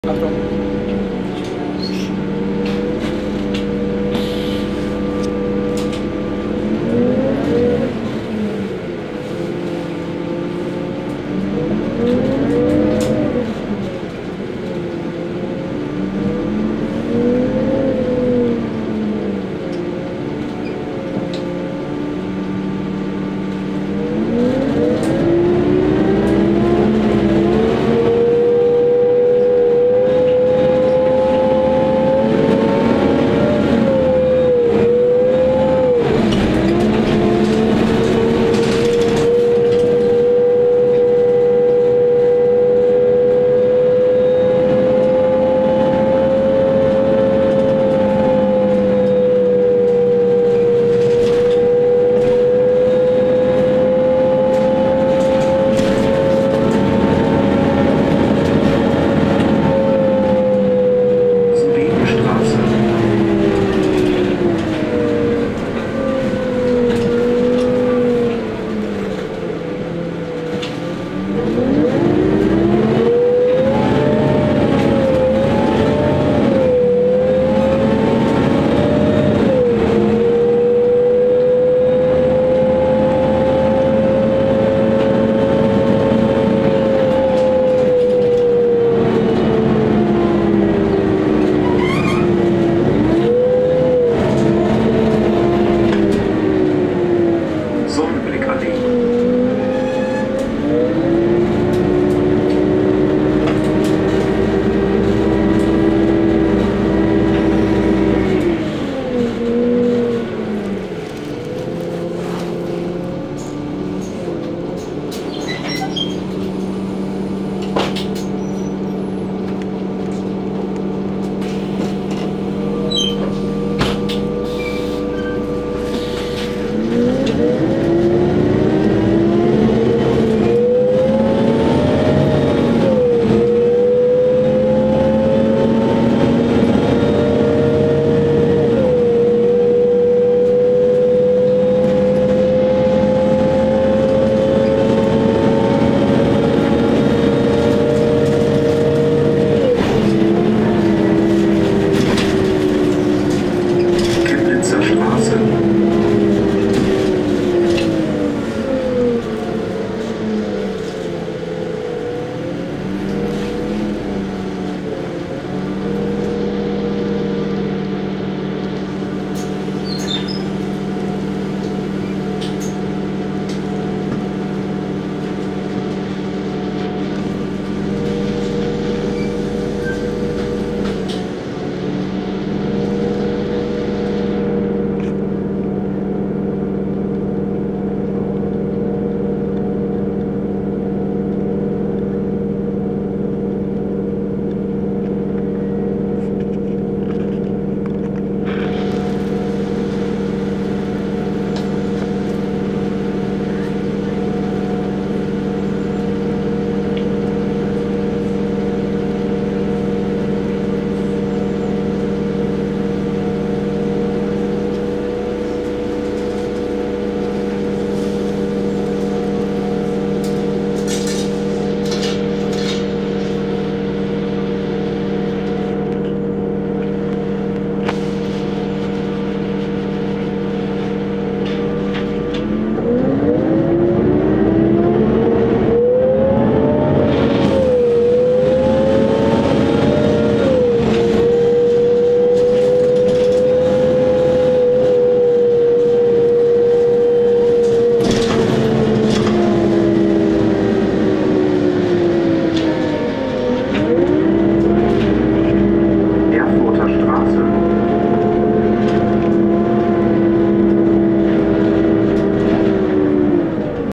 MR-VB 16 - Mercedes Citaro CNG - Erdgas-Bus in Marburg am Richtsberg (Mitfahrt)
Mitfahrt im Neufahrzeug aus dem Jahrgang 2005. Dieser Mercedes Citaro (O 530)  Erdgas-(CNG)Bus fährt in diesem Video vom Eisenacher Weg bis zur Erfurter Straße in Marburg. Auf der Linie 4 war es an diesem Sonntag relativ ruhig!